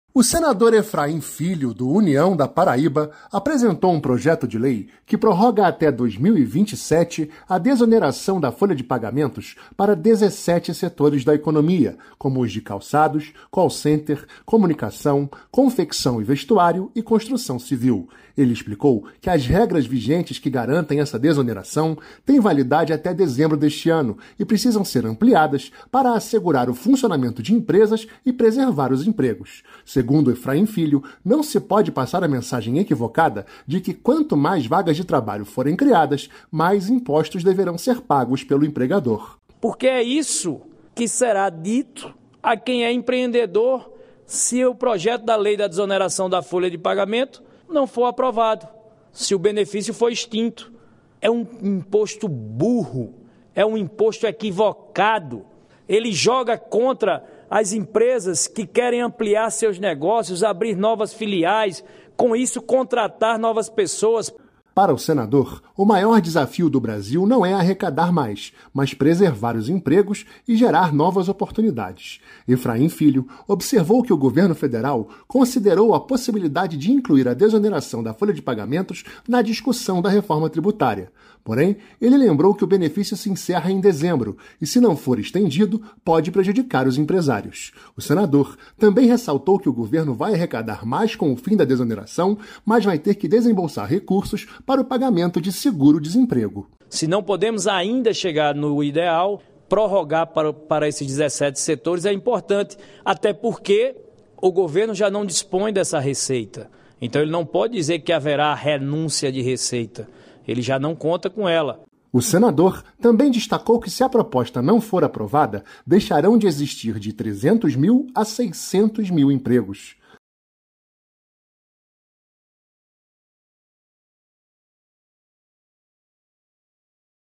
Desoneração da folha de pagamentos até 2027 é tema de projeto em análise no Senado
A desoneração da folha de pagamentos de 17 setores, como o de calçados, comunicação e construção civil, pode ser prorrogada até o final de 2027. Esse é o objetivo de um projeto de lei (PL 334/2023) apresentado pelo senador Efraim Filho (União-PB). Para ele, o fim do benefício, previsto para ocorrer em dezembro deste ano, pode ocasionar o fechamento de até 600 mil postos de trabalho.